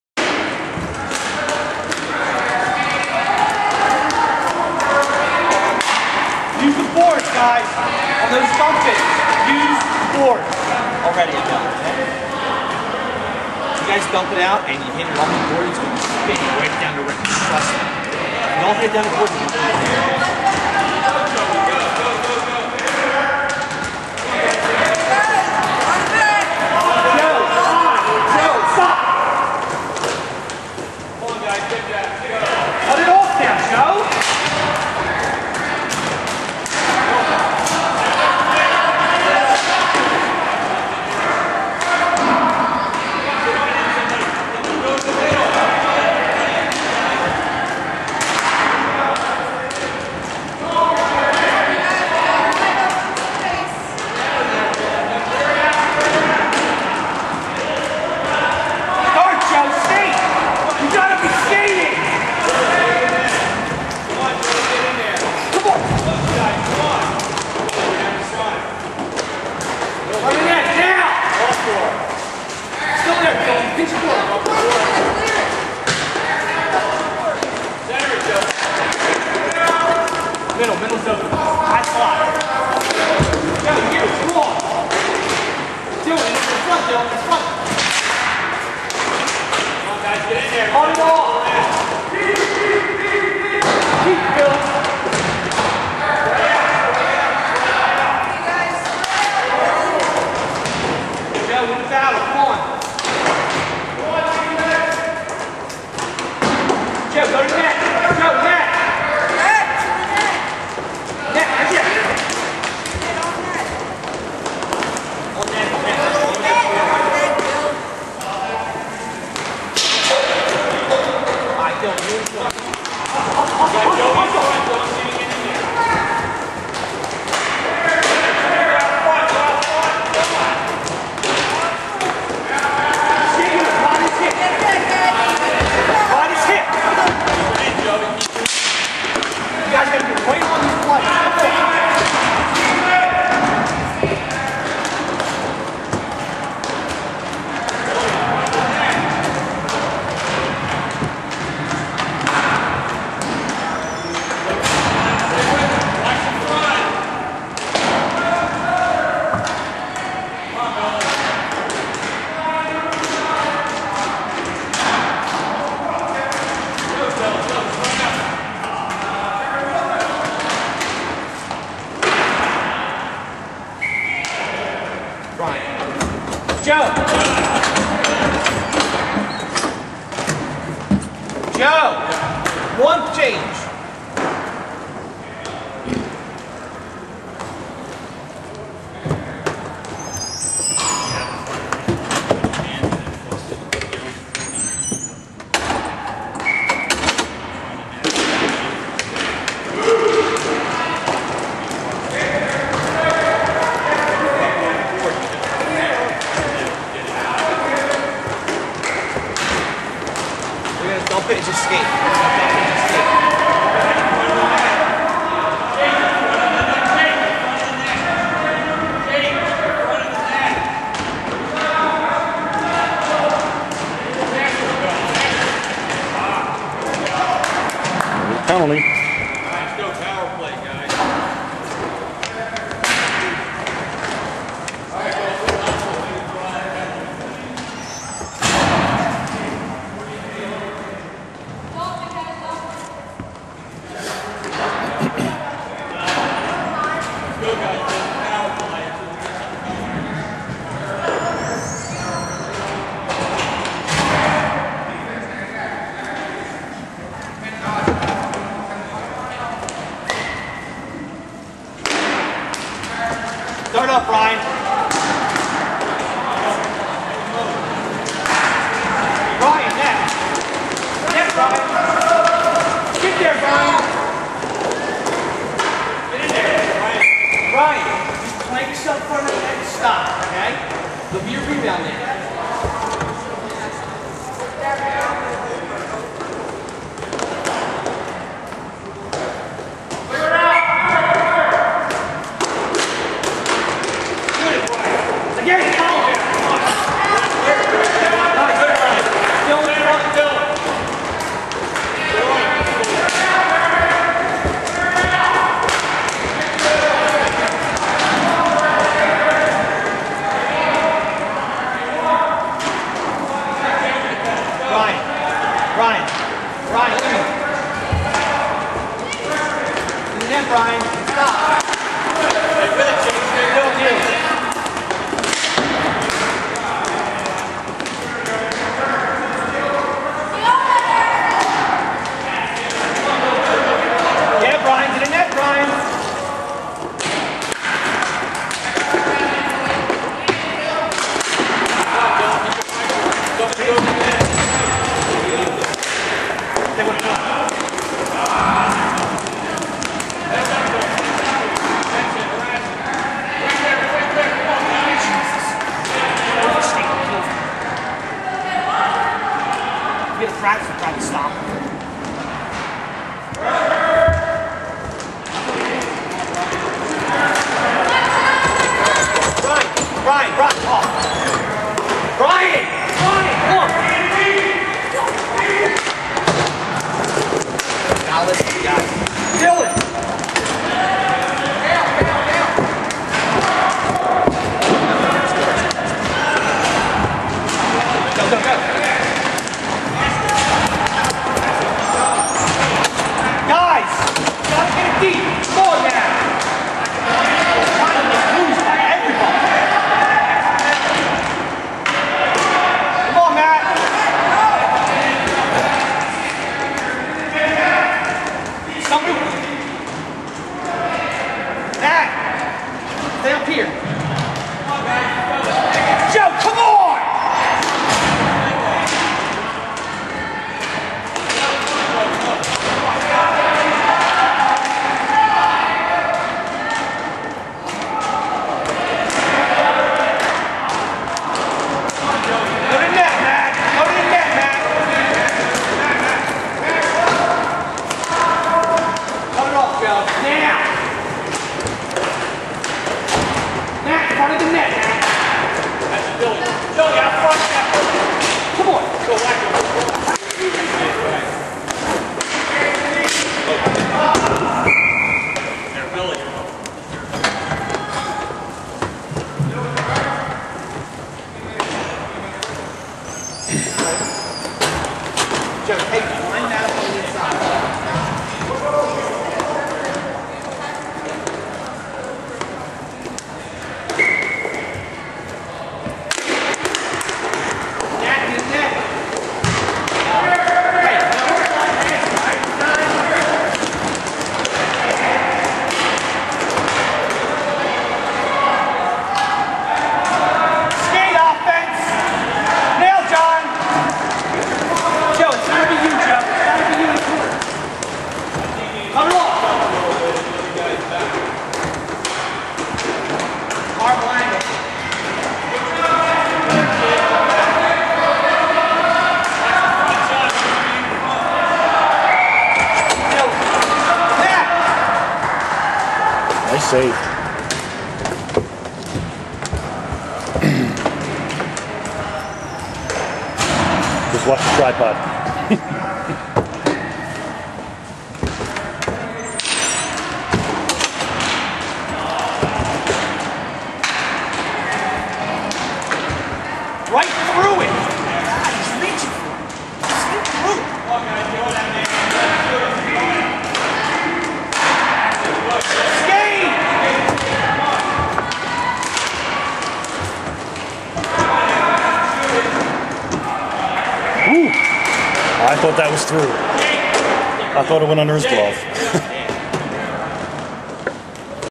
Period 1, Part 1 of 3 - U12 Roller 01022010
U12 Roller Hockey Championship game between Orange and Purple teams at Flyers SkateZone in Voorhees, NJ